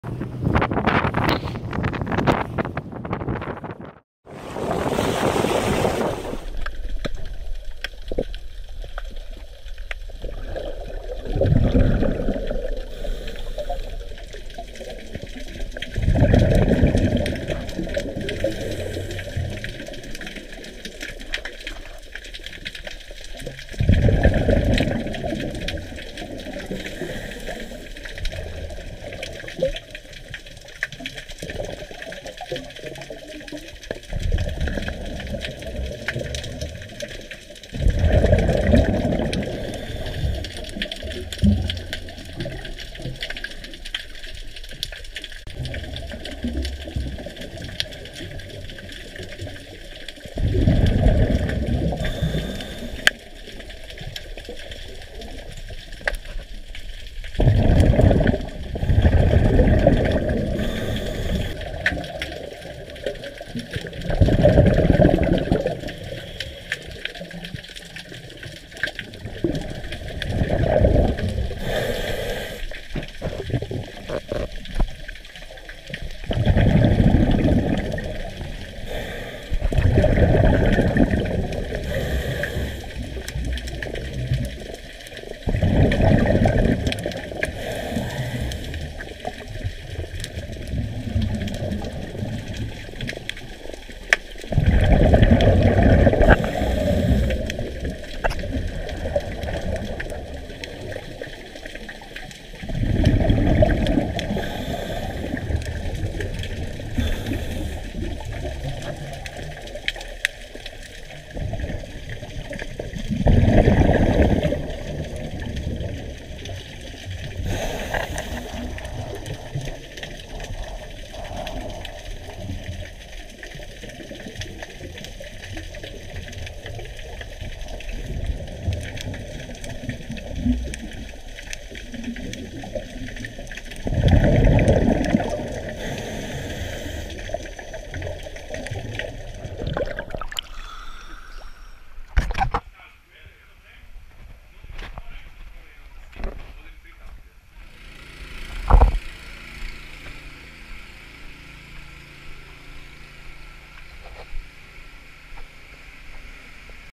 Banana Islands / Sierra Leone / Polish Diving Expedition 2016
If you are thinking about silent place, with tasty cuisine (fishes, lobsters and so on) Banana Islands will be the perfect place for you.

The owner is helpful and pleasant. You will not regret!